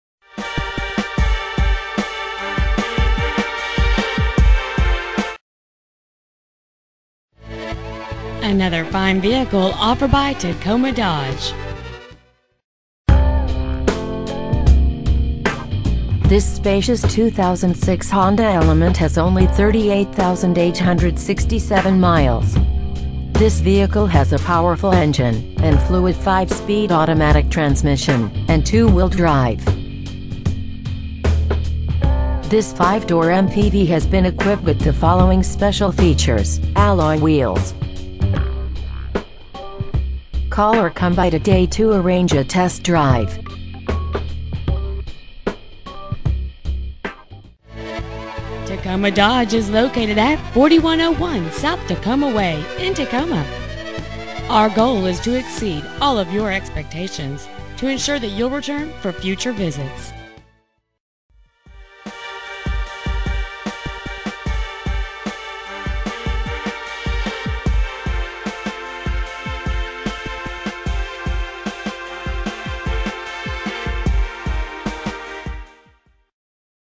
2006 Honda Element Tacoma WA 98409
Tacoma Dodge
 4101 S. Tacoma Way
 Tacoma, WA 98409

 Year: 2006
 Make: Honda
 Model: Element

 Trans.: 5 Spd Automatic
 Exterior: Silver
 Miles: 38,867
 Interior: Grey